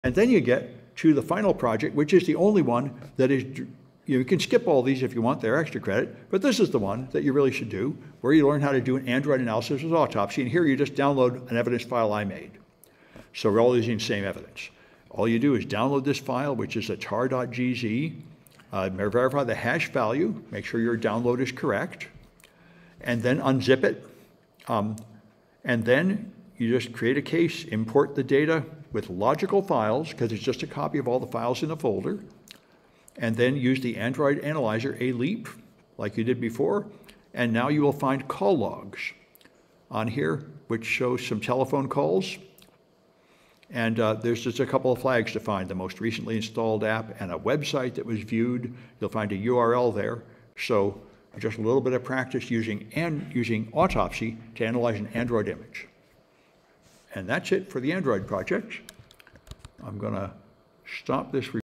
M 144 Android Analysis with Autopsy
A lecture for a Computer Forensics class